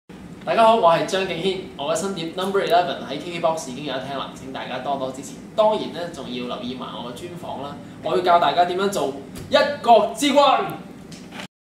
張敬軒要教大家做王上?
KKBOX探訪隊日前有幸面聖，會見軒仔皇上做專訪，想知道有關軒仔皇上新碟之事及如何日理萬機，請密切留意黎緊的KKBOX專訪喇！